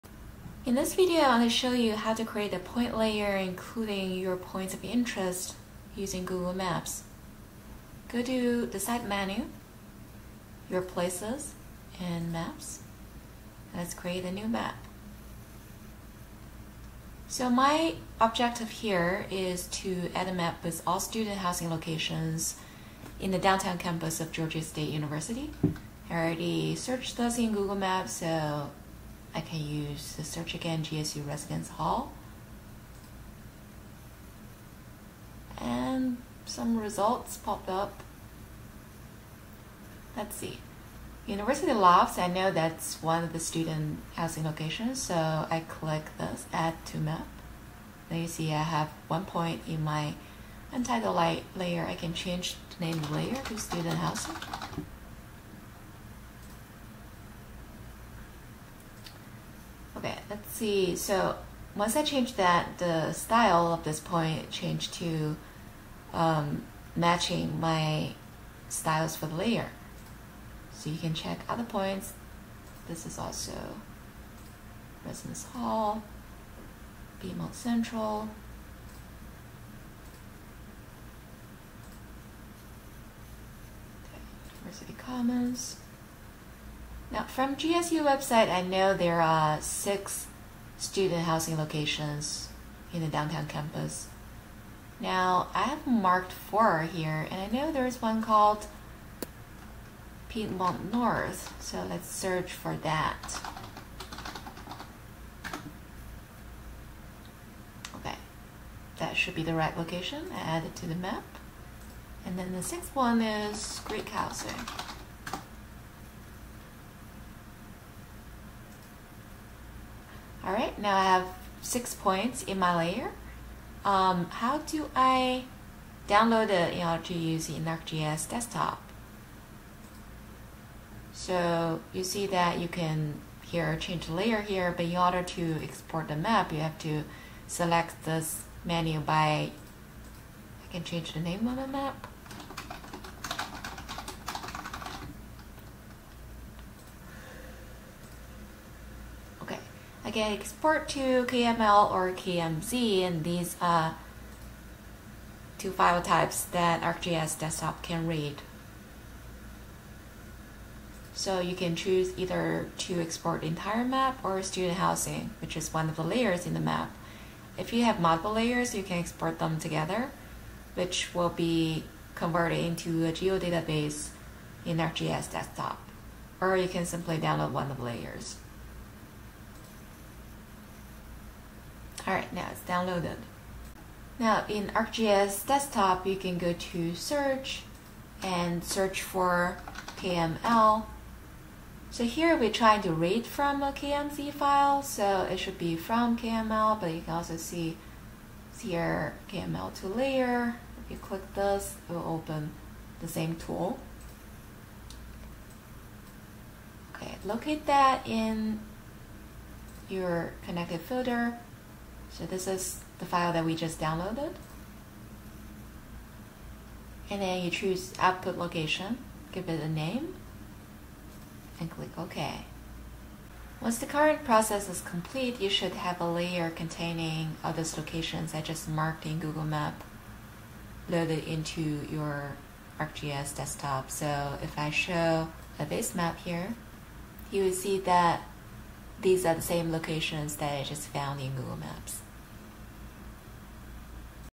Create a POI layer with Google Maps
You can create a map of points of interest (POI) in Google Maps and export it to a format that can be used in ArcGIS Desktop.